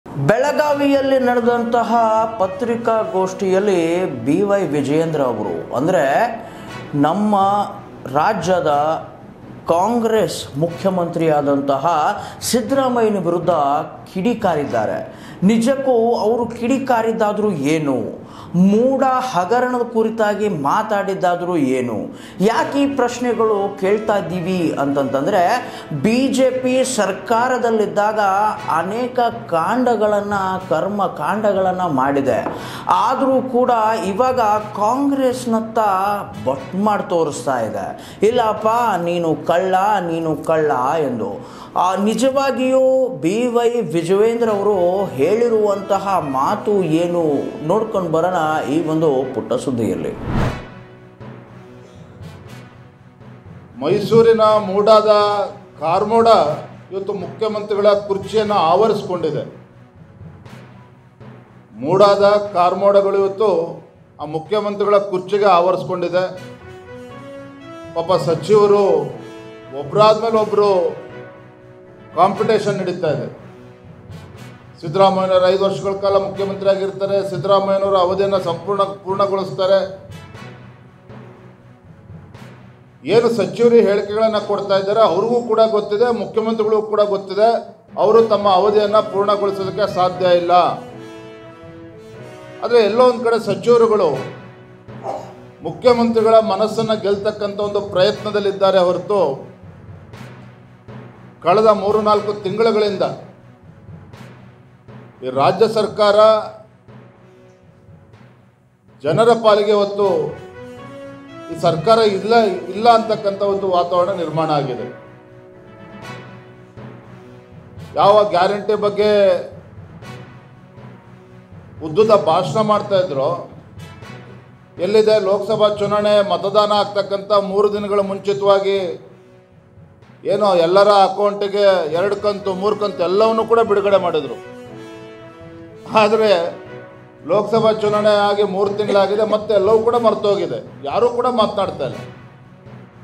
CM Siddaramaiah : ಮುಡಾ ಕಾರ್ಮೋಡ ಸಿಎಂಗೆ ತಗುಲಿದೆ ಬೆಳಗಾವಿಯಲ್ಲಿ ವಿಜಯೇಂದ್ರ ಹೇಳಿಕೆhsrnewslive
Karnataka | ಕರ್ನಾಟಕ
Kannada | ಕನ್ನಡ